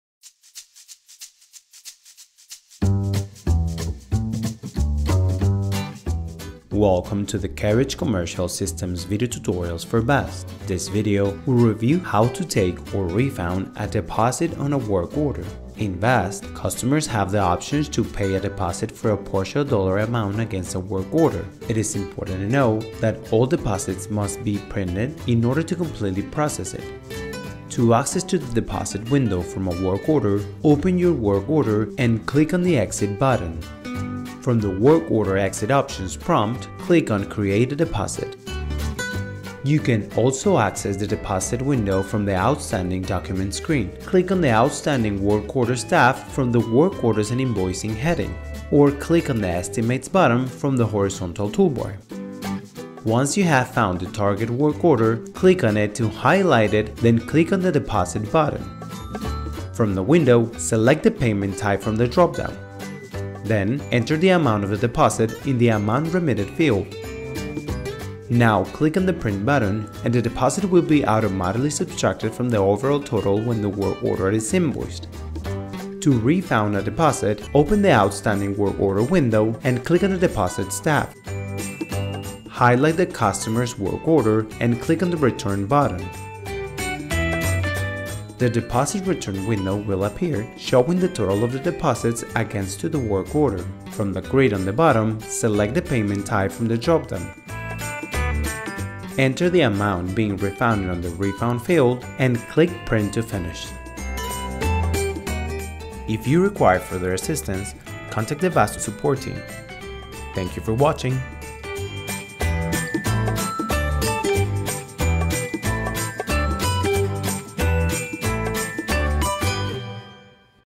Applying and Refunding Deposits
This Video will review how to take or refund a deposit on a Work Order.